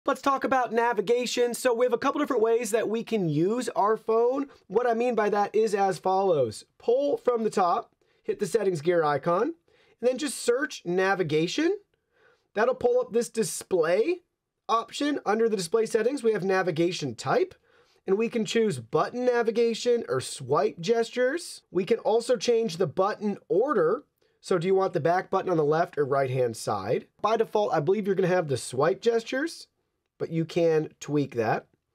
Get Started with the Samsung Galaxy A53 5G: Device Navigation Guide
Are you looking for the ultimate guide to navigating your new Samsung Galaxy A53 5G? Look no further! In this video, I'm going to show you step-by-step how to unlock the full potential of your device. From setting up your phone, to mastering the basics of navigation, I'll cover it all. Discover how easy it is to get started with your new Samsung Galaxy A53 5G today!

SAMSUNG Galaxy A53 5G A Series Cell Phone, Factory Unlocked Android Smartphone, 128GB, 6.5” FHD Super AMOLED Screen, Long Battery Life, US Version, Black